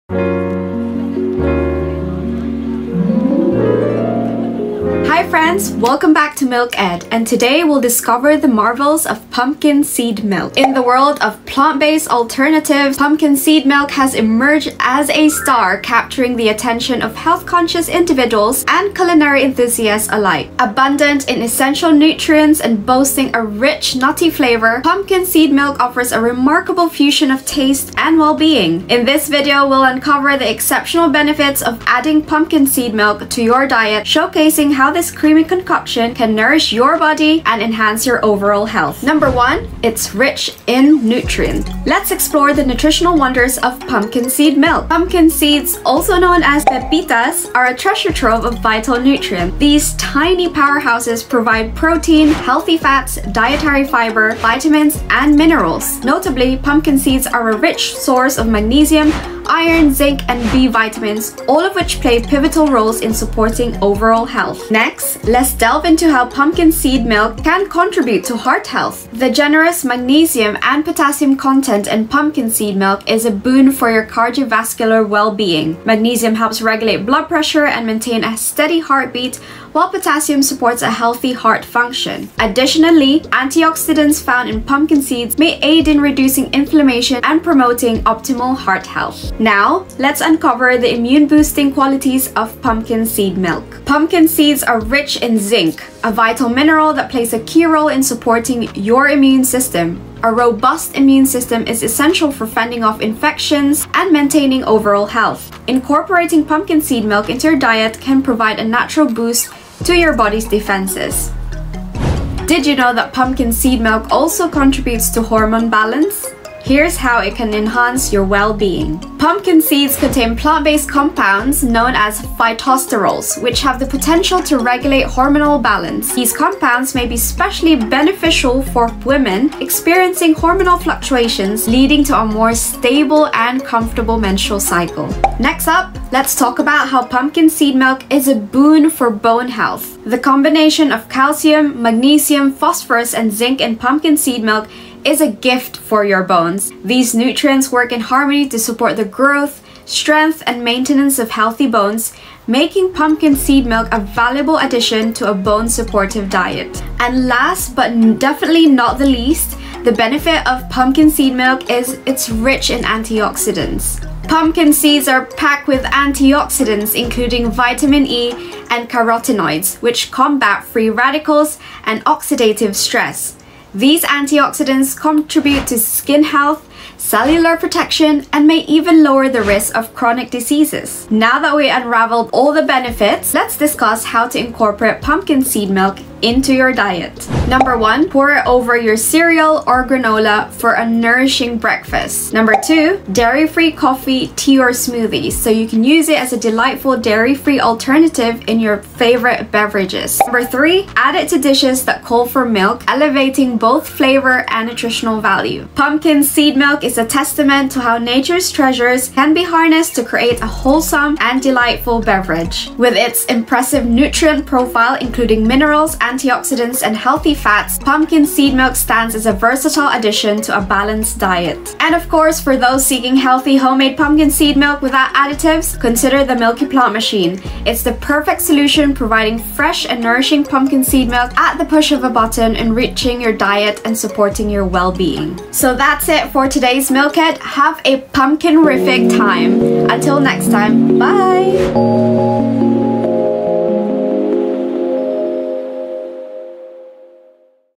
Pumpkin Seed Milk Benefits Unveiled: Why It’s Hailed as the Elixir for Health & Wellness? 🎃
In this video, we're diving deep into the world of Pumpkin Seed Milk and uncovering its incredible benefits. 🎃🥛

Pumpkin Seed Milk is not only a delicious dairy-free alternative but also a powerhouse of nutrition. We'll explore its various advantages, including:

🌟 Rich in Essential Nutrients: Discover the vitamins, minerals, and antioxidants that make pumpkin seed milk a nutritional treasure.

🌱 Plant-Powered Goodness: Learn why this milk is an excellent option for those seeking a dairy-free, vegan, or plant-based diet.

💪 Health Benefits: Uncover how pumpkin seed milk can support your immune system, heart health, and more.

🧡 Nutty and Delicious: Find out just how delightful and versatile this milk is in your favorite recipes.

Whether you're lactose intolerant, vegan, or simply looking to expand your culinary horizons, pumpkin seed milk is a fantastic addition to your kitchen.

Join us as we explore the benefits and versatility of this nutty elixir, and get ready to elevate your health and taste buds to a whole new level. 🎥👩‍🍳🍂